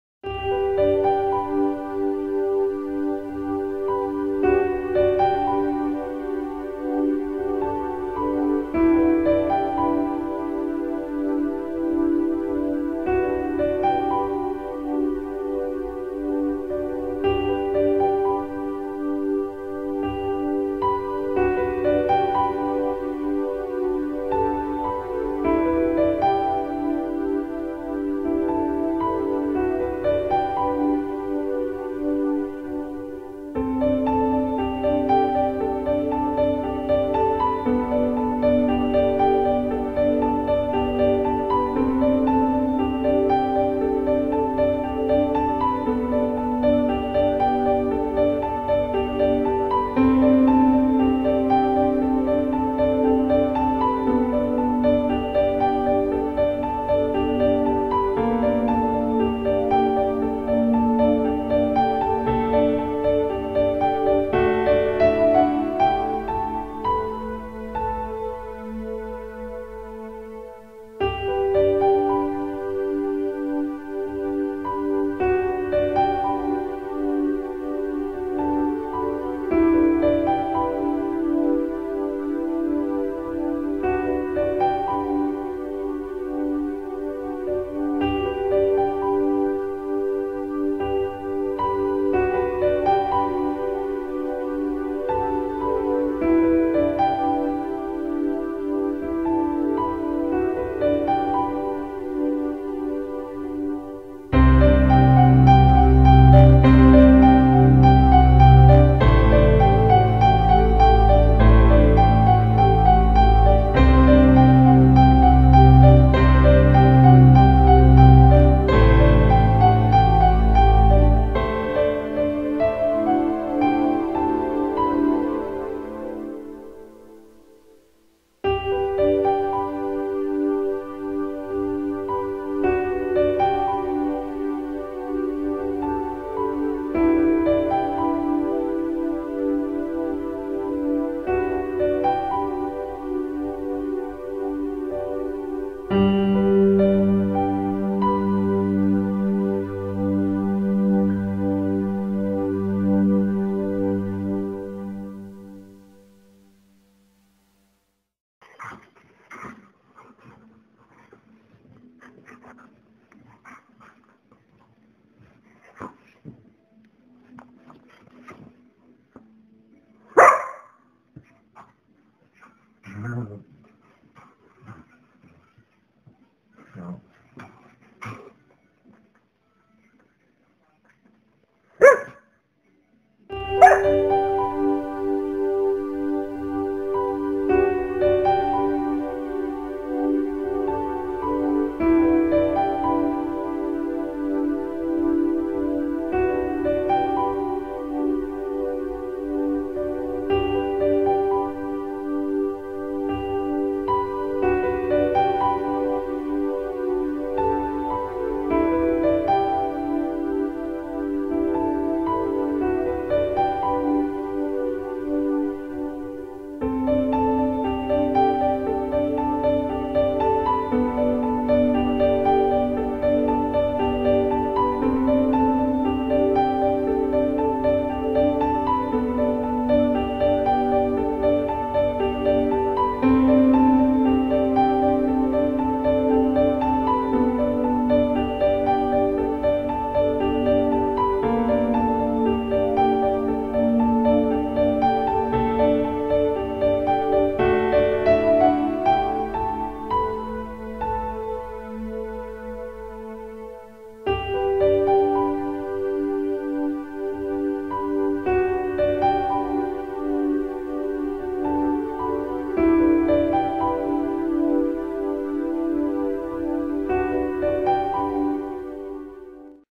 Bluto's Story and Fundraiser Video
This video is about Bluto, one of the greyhounds up for adoption with All About Hounds located in Middle Tennessee.  Bluto has a very painful eye condition that requires surgery.  This video was made to tell his story and as a fundraiser.